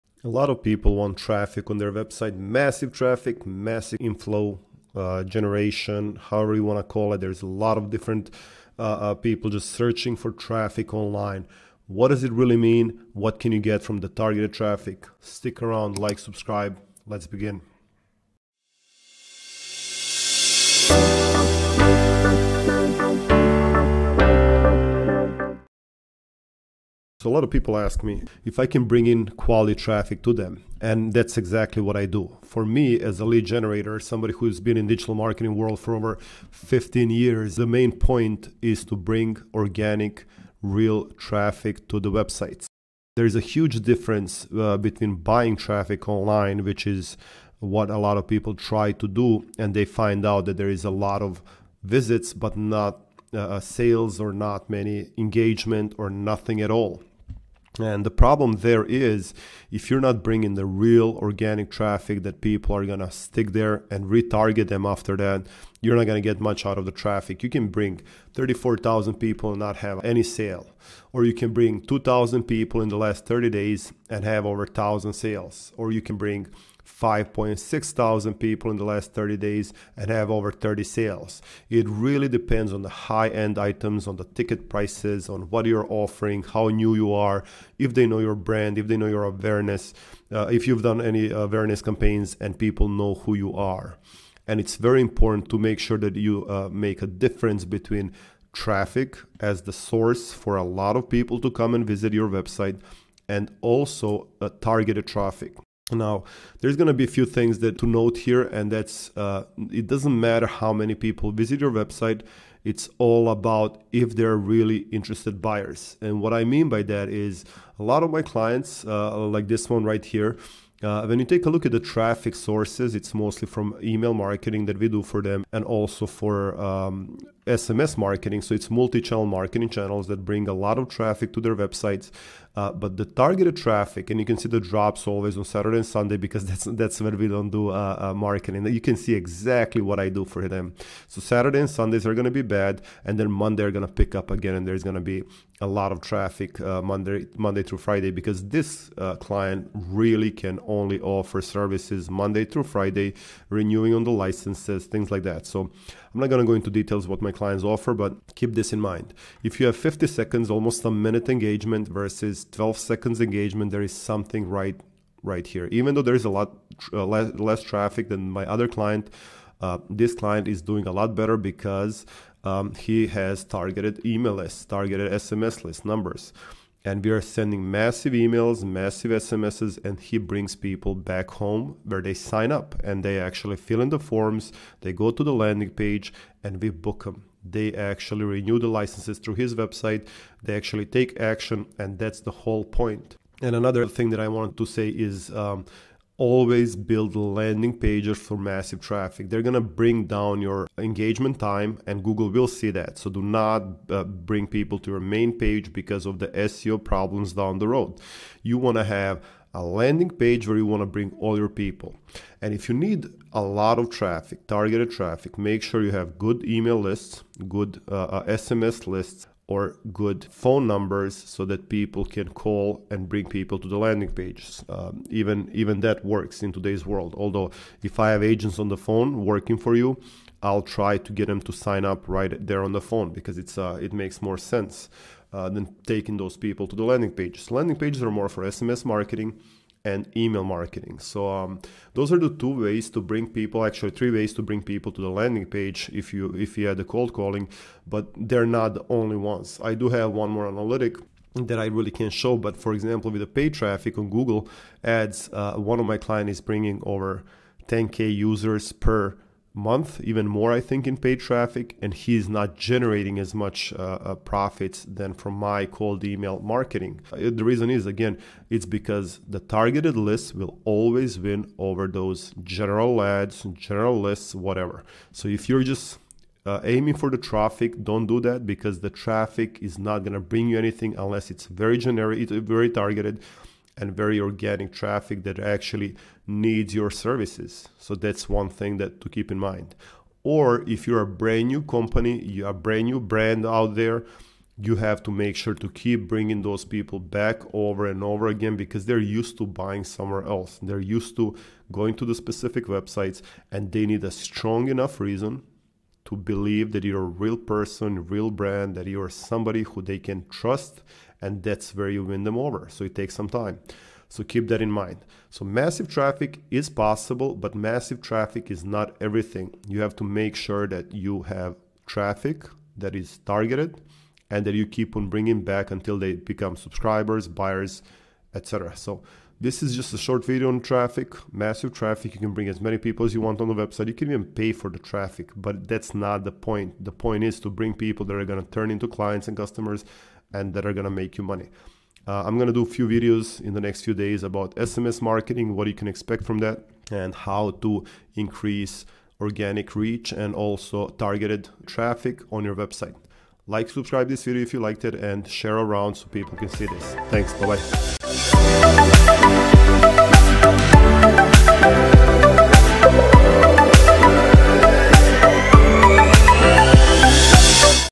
How to get massive website traffic
If you were ever wondering what mass website traffic can do for you, here is your answer:
Some businesses skyrocket, while others are just wasting their money. There are a few things that you need to know and rules you follow to make sure you get the most out of your website traffic.